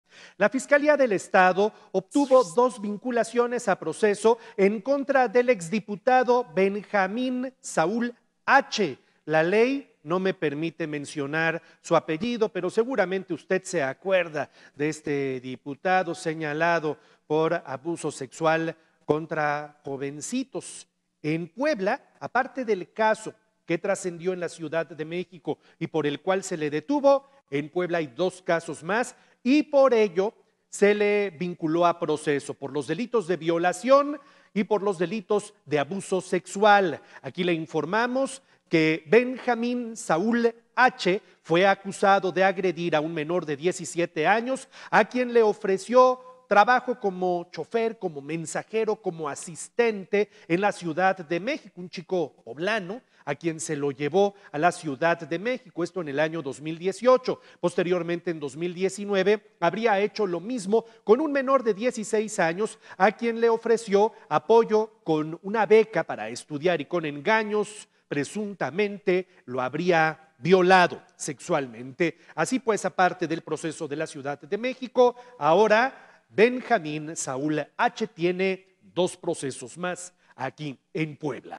Vinculado exdiputado Saúl || Noticias con Juan Carlos Valerio
La Fiscalía General del Estado de Puebla obtuvo dos vinculaciones a proceso en contra del exdiputado Saúl H.